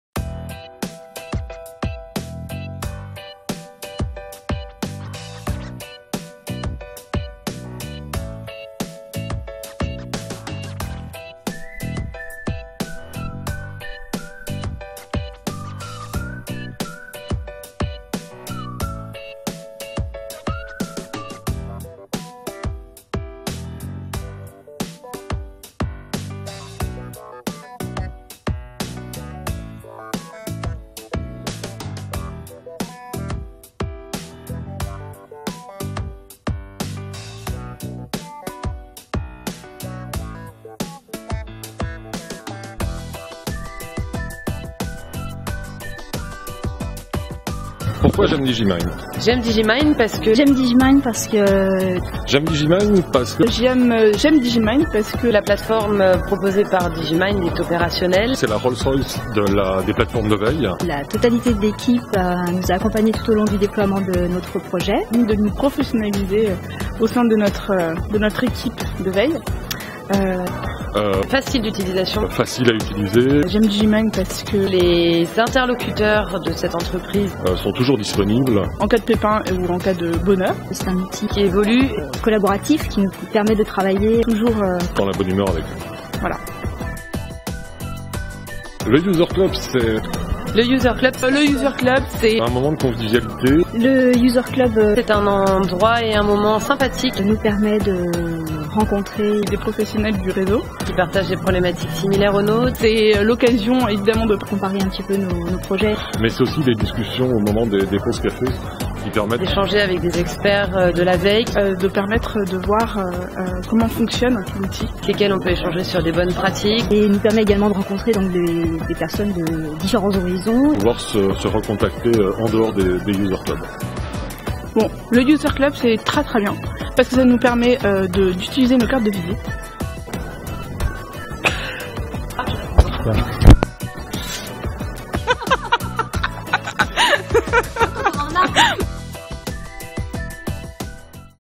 Digimind User Club 2014
Jeudi 6 mars dernier s'est tenu le 1er User Club Digimind de l'année 2014 à l'Hôtel particulier Eurosites. L'occasion pour Transdev de témoigner de leur utilisation de la solution Digimind Intelligence.

Le début de la matinée a été consacrée aux actualités et nouveautés 2014 de Digimind puis Transdev, un des leaders mondiaux des transports publics, a présenté son projet WebVibe, mis en place à partir de la solution Digimind.

Pendant la pause, nous avons pu filmer des témoignages des utilisateurs de la plateforme Digimind. Découvrez dans cette vidéo ce que nos clients pensent de notre outil!

Thursday 6th March saw the first Digimind User Club of 2014 at the Hôtel Particulier Eurosites. It was a chance for Transdev to discuss how they used the Digimind Intelligence tool to their advantage.

The opening presentation looked at Digimind updates and new features for 2014, followed by a presentation by Transdev - one of the world's leading public transport companies - on their WebVibe project, implemented with the help of Digimind's solutions.

During the coffee break, we had the opportunity to gather some feedback from user of the Digimind platform. Watch the video for a glimpse into what our clients think of our tool!